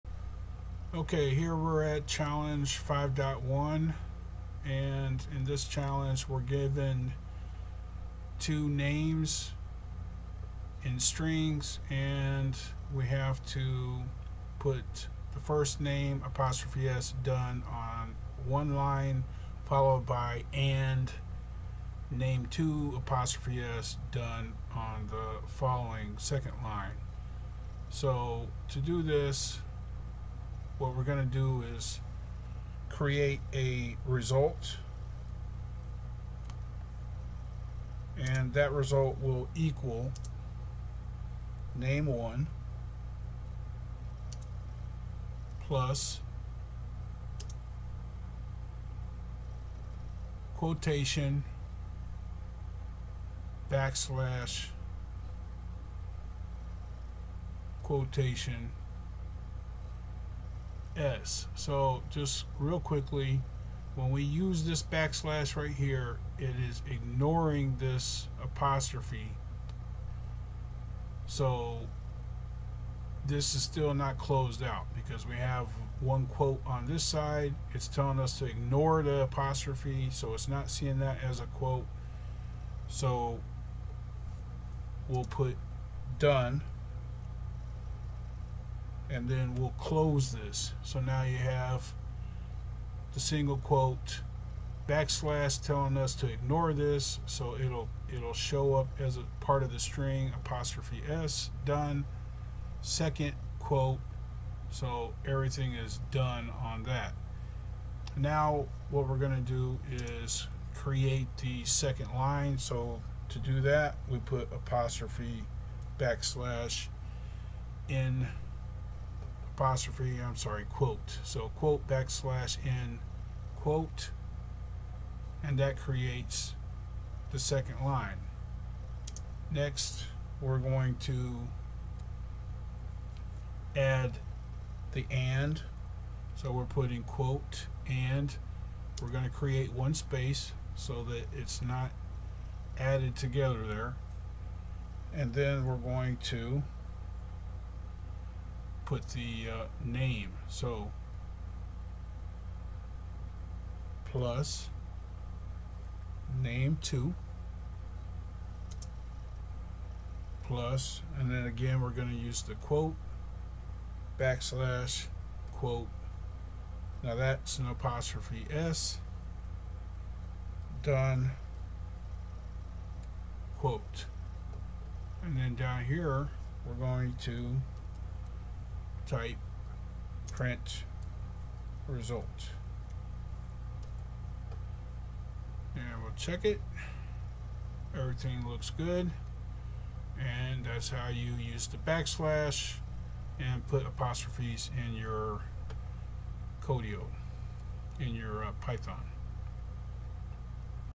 IT-140 Introduction to Python Codio Challenge 5. 1.
Explanation of Codio challenge 5. 1. the use of the \ Backslash as an escape in Python